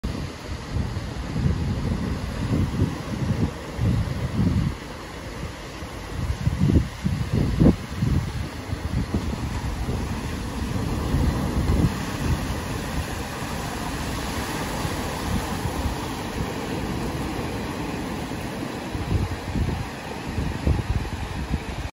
Veraval Chaupati Beach
Beautiful sea shore of Veraval Chaupati. Peace time and me time